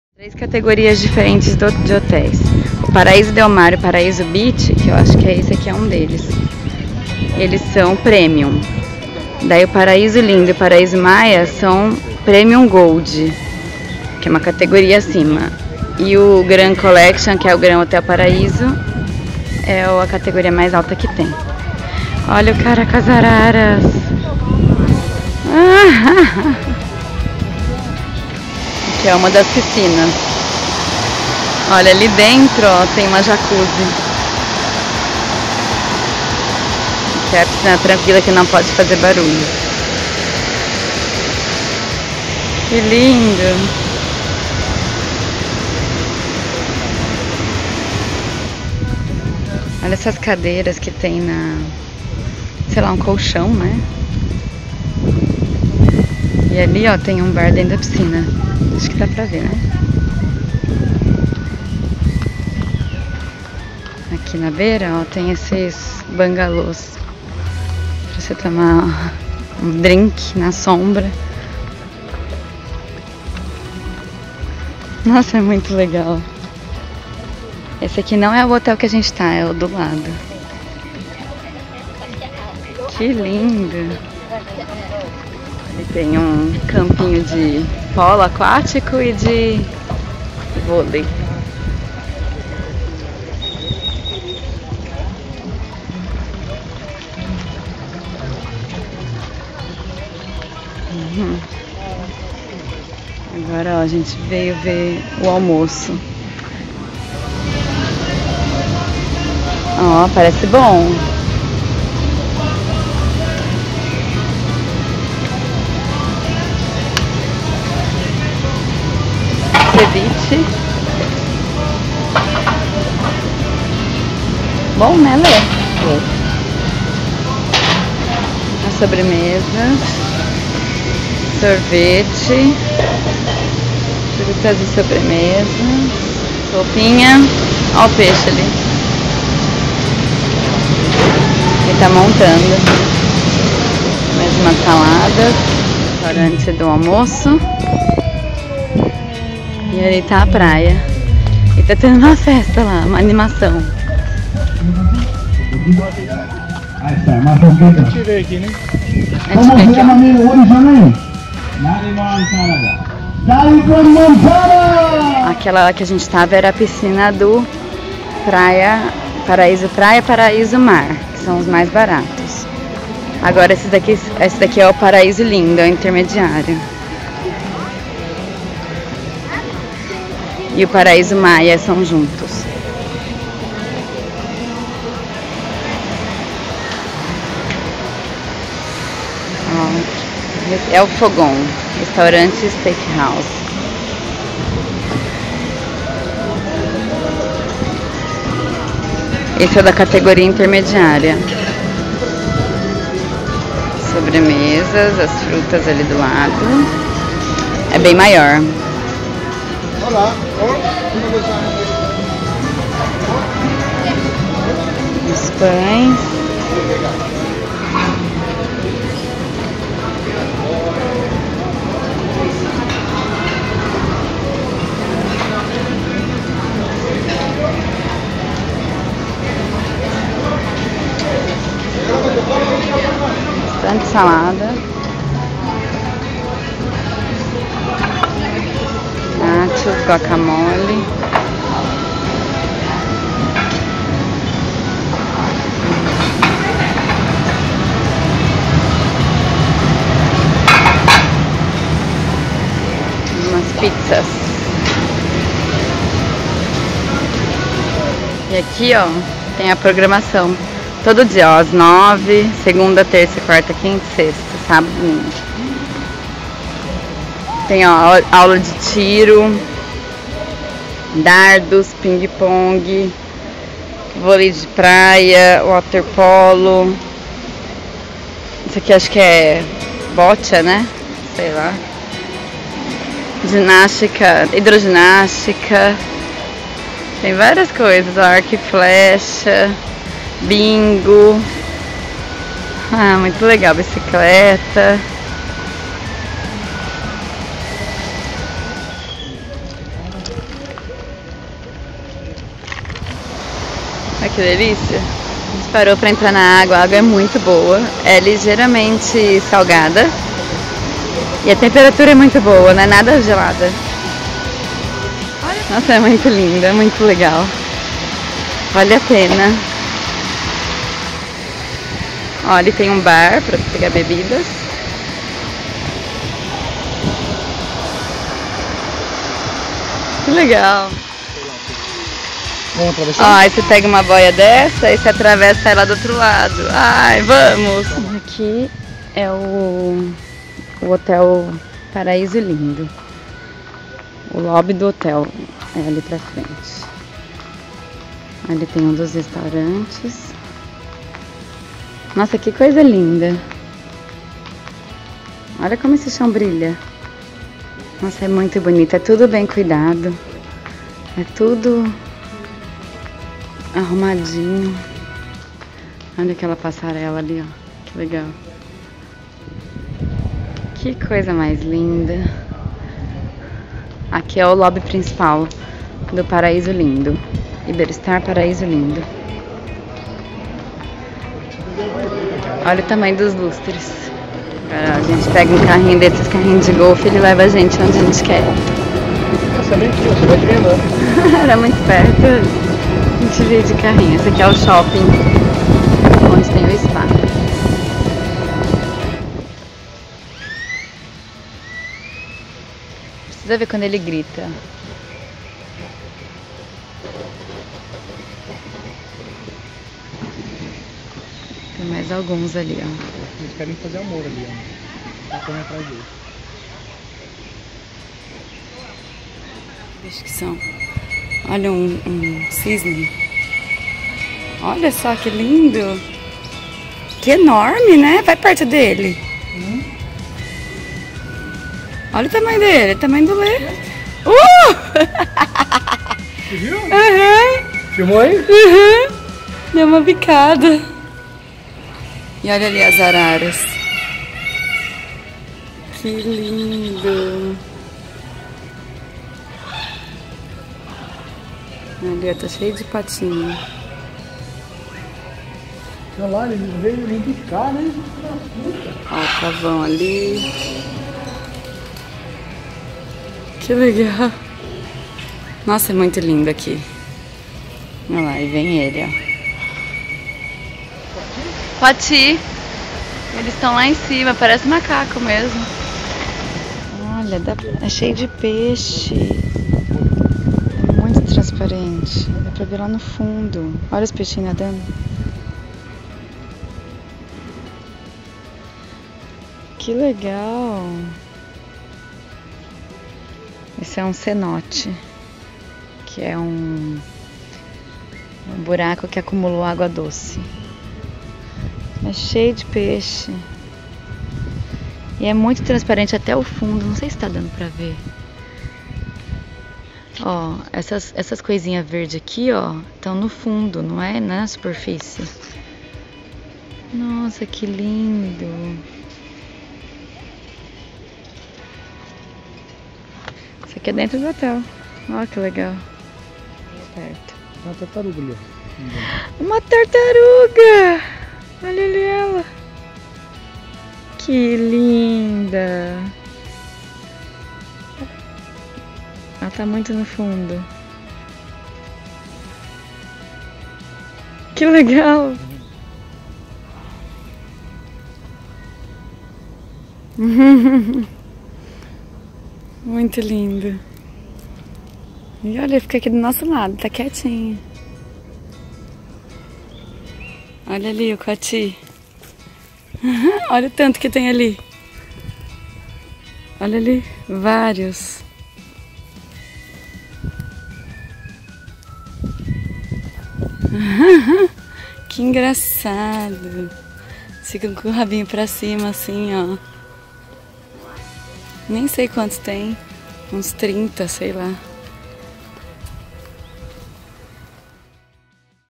Tour pelo Complexo Iberostar em Cancun (Playa del Carmen)
O complexo Iberostar em Playa del Carmen tem 5 hotéis de tres categorias diferentes. Nós estávamos hospedados no Grand hotel Paraíso que é da categoria Grand Collection. Além desse tem o de categoria premium gold, que é a intermediária e o mais simples que é categoria premium. Todos tem ótimas piscinas e área de lazer linda, a grande diferença é a comida e o quarto. No Grand hotel paraíso você tem à disposição concierge para agendar os restaurantes que você queira ir, lagosta no buffet, carangueijo, camarões enormes e hambúrguer de Kobe beef. Os quartos todos tem banheira e são muito luxuosos e confortáveis. Não conheci os quartos dos outros hotéis, mas conheci o restaurante e acho que isso é que faz mais diferença entre as categorias.